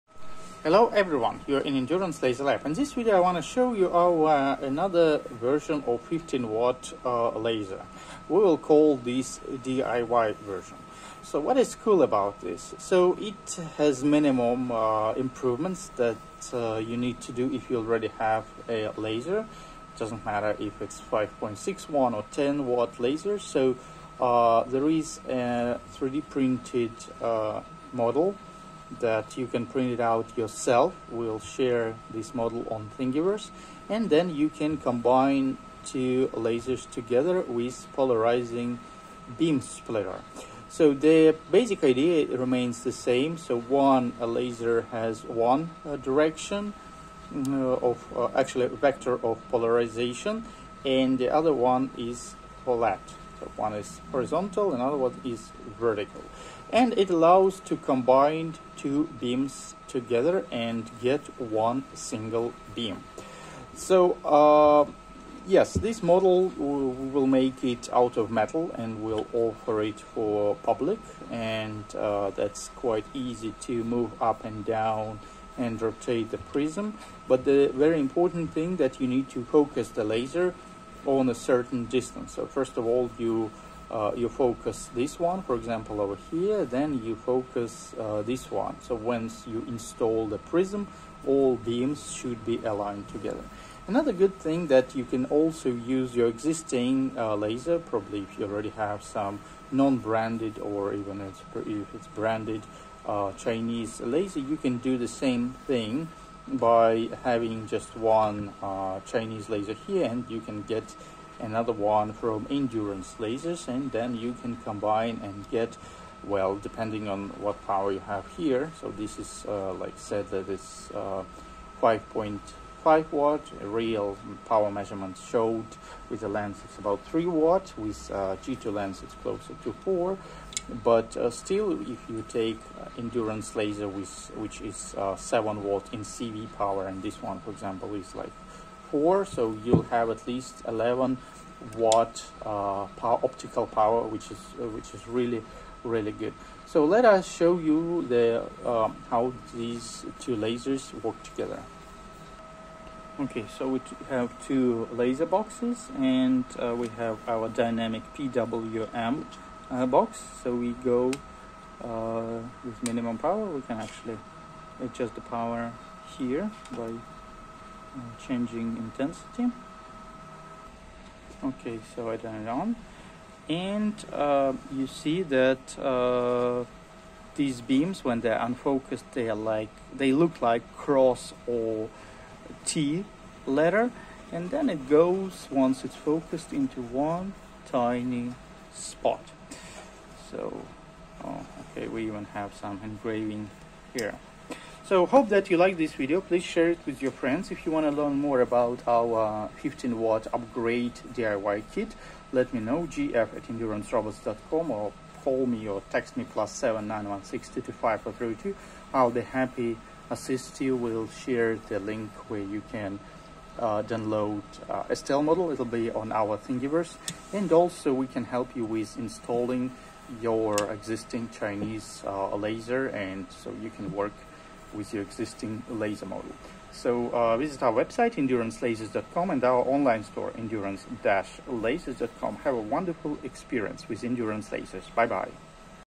An Endurance 15 watt (15000 mw) DIY laser upgrade. Combine 2 lasers together!
Upgrade your current laser with an Endurance upgrade kit.

comments and questions are welcome!